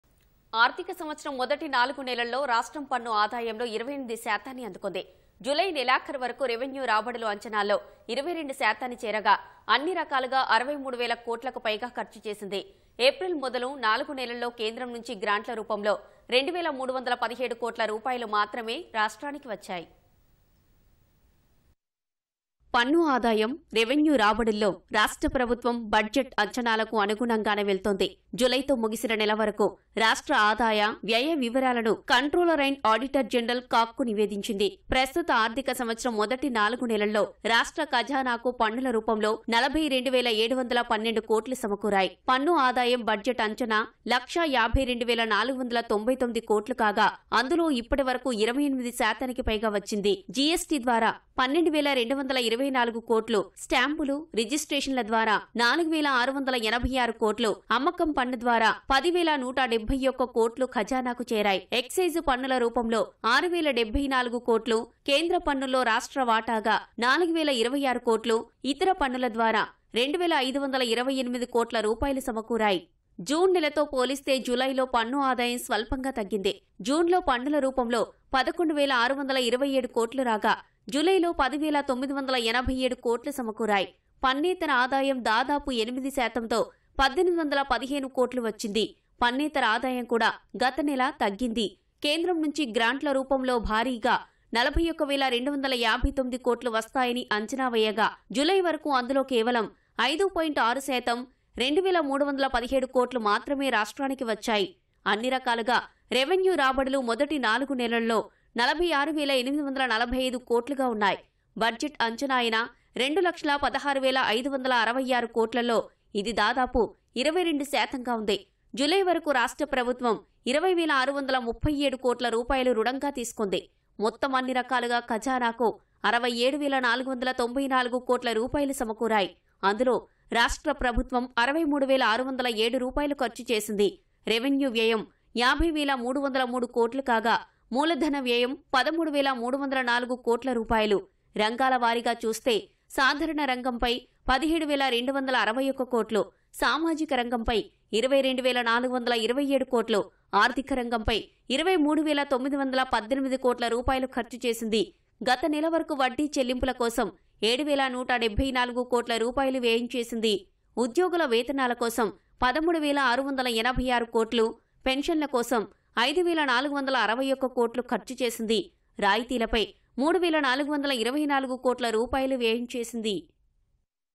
Collection of Revenue & Tax Income | Telangana Reaches It's Goal | అంచనాలకు అనుగుణంగానే పన్ను ఆదాయం
ఆర్థిక సంవత్సరం మొదటి నాలుగు నెలల్లో రాష్ట్రం పన్ను ఆదాయంలో 28 శాతాన్ని అందుకుంది. జులై నెలాఖరు వరకు రెవెన్యూ రాబడులు అంచనాల్లో 22 శాతాన్ని చేరగా... అన్ని రకాలుగా 63 వేల కోట్లకు పైగా ఖర్చు చేసింది. ఏప్రిల్ మొదలు నాలుగు నెలల్లో కేంద్రం నుంచి గ్రాంట్ల రూపంలో 2 వేల317 కోట్ల రూపాయలు మాత్రమే రాష్ట్రానికి వచ్చాయి.